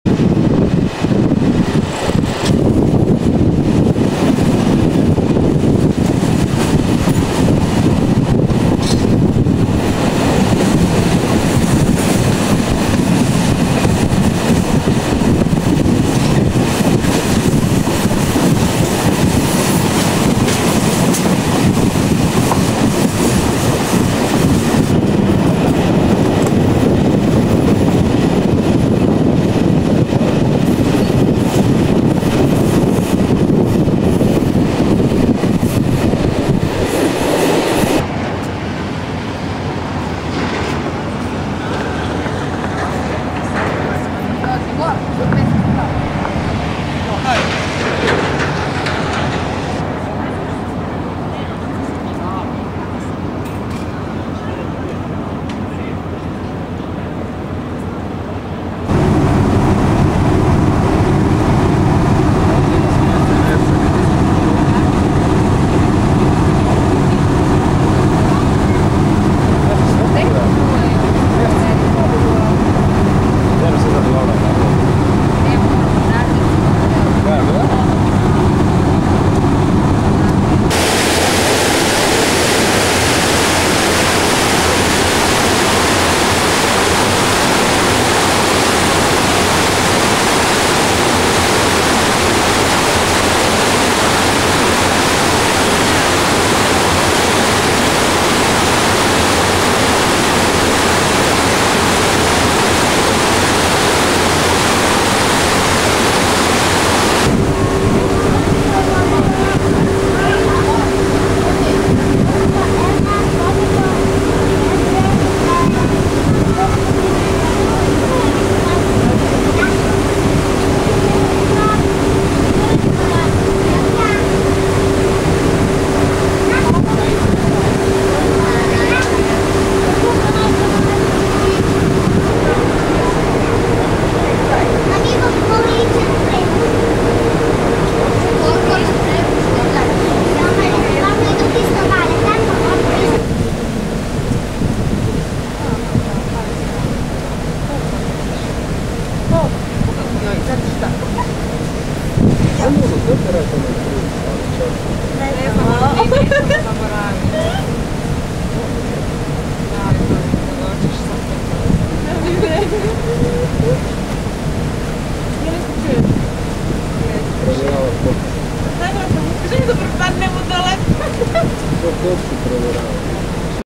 Rijeka to Zadar via Jadrolinija
This starts off with the train approaching Rijeka and continues with a Jadrolinija ferry ride from Rijeka to Zadar.